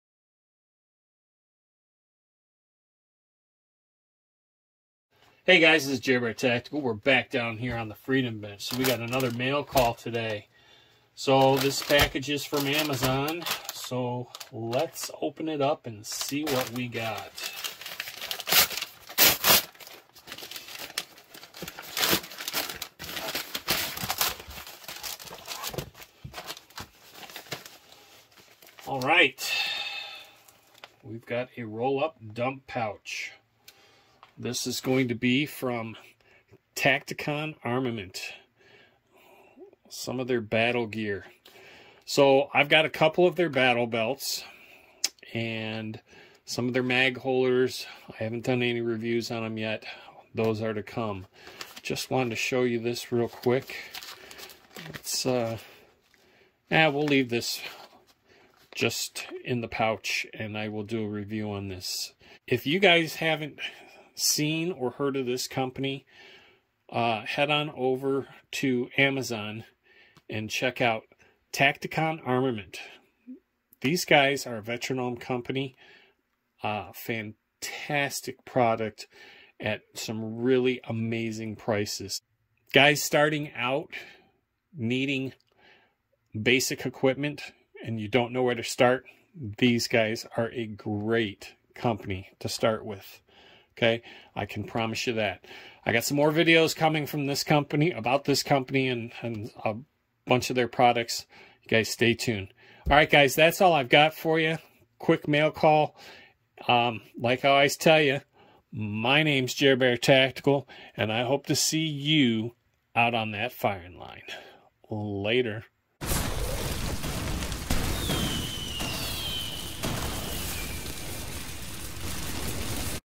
Mail Call from Amazon … 11/03/2022
Hi Guys, today's video is a Mail Call from Amazon. I picked up a roll-up dump pouch from Tacticon Armament to add to my battle belt for empty mags. This is a Great company with lots of budget friendly gear. Amazon Link for the dump pouch